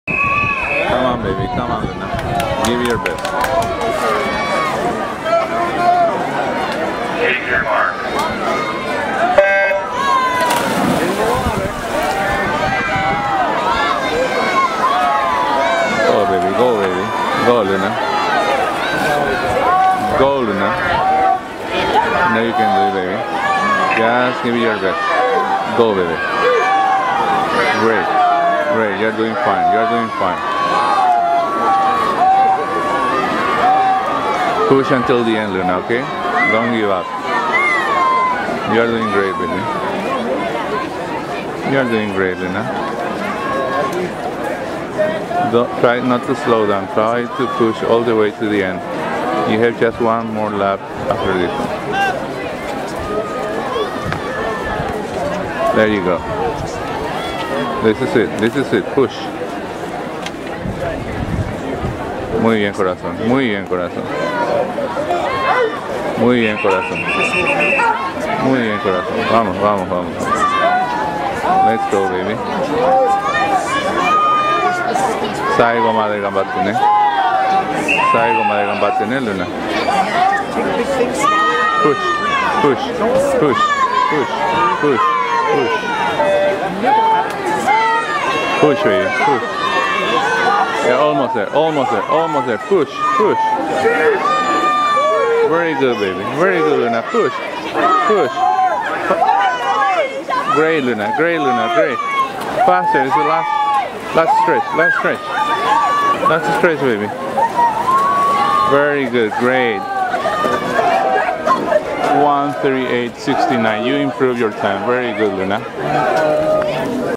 2014 PC FW - L's 100 Breaststroke Preliminary 8/3/2014
L missed qualifying for the finals by 4 seconds.